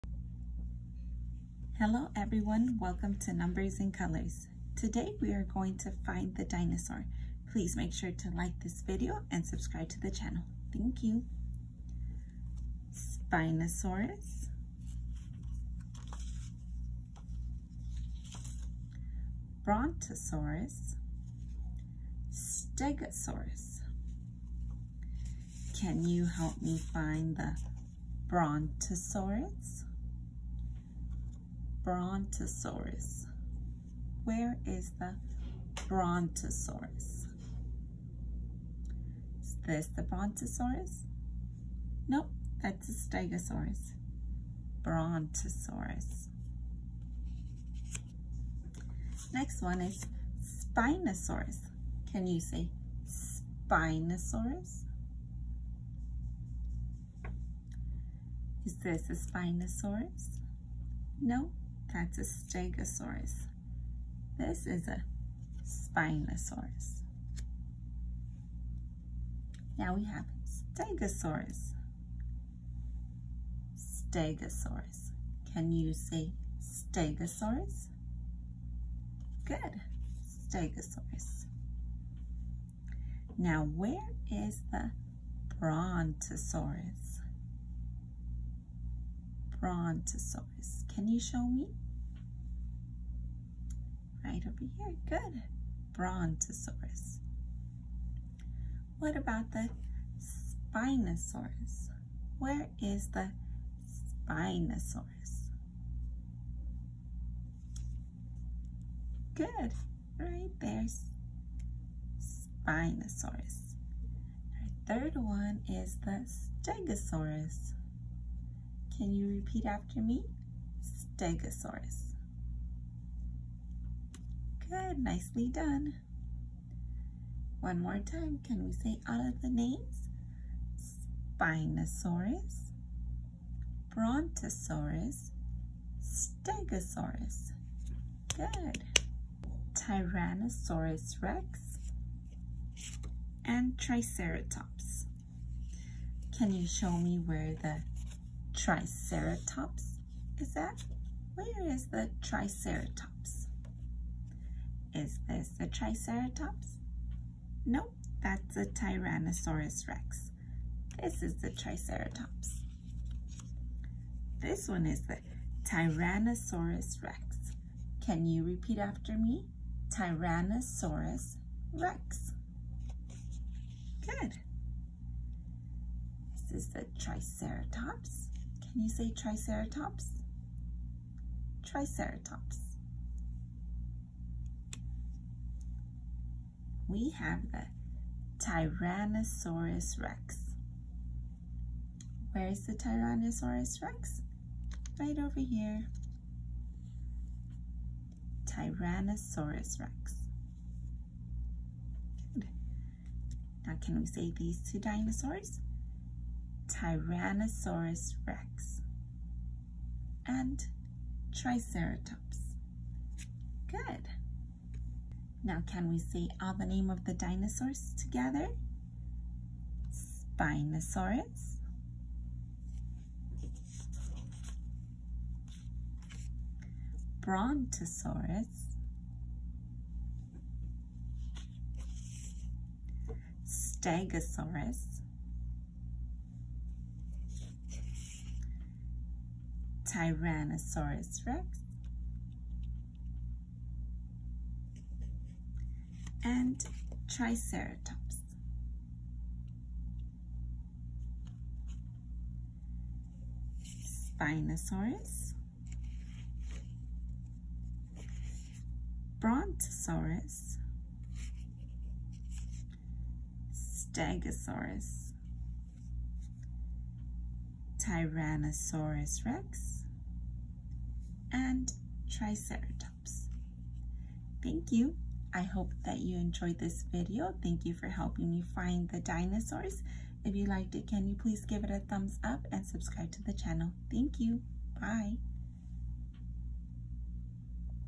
Find the Dinosaur | Dinosaurs | Triceratops | Stegosaurus | Kids Educational Videos 🎨📚🦖🦕
Hello everyone! Welcome to numbers and colors. Today we are going to find the dinosaur. 🎨📚🖍🦕🦖